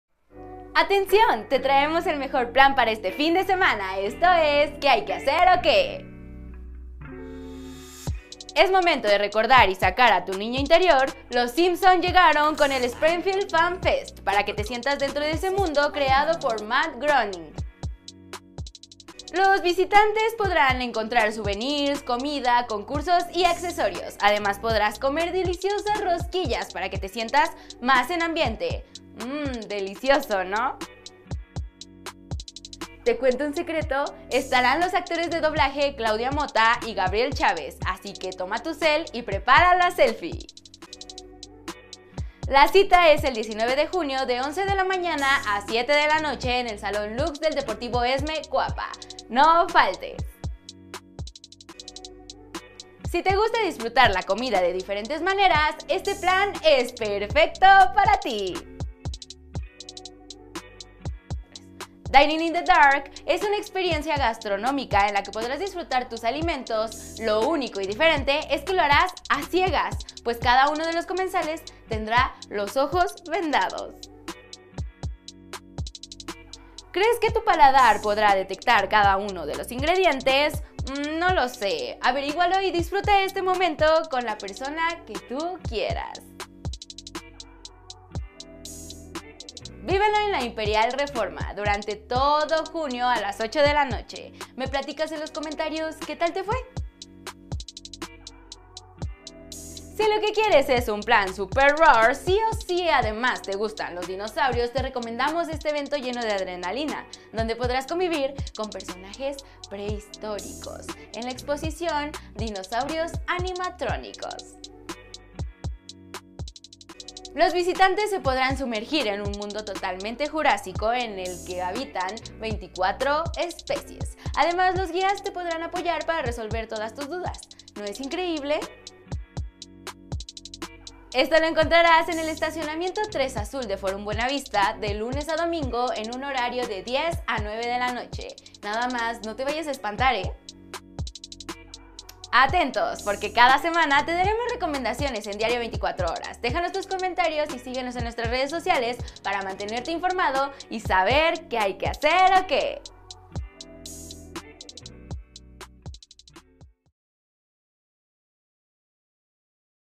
QuéHayQueHacerOQué | Llega la exhibición Dinosaurios Animatronic
QuéHayQueHacerOQué... ⁉ | ¡Ay, caramba! El Fan Fest de Los Simpson llega a la CDMX 😍 o si lo prefieren ven a viajar a 65 millones de años en el pasado a la exhibición 'Dinosaurios Animatronic. Un Nuevo Mundo'.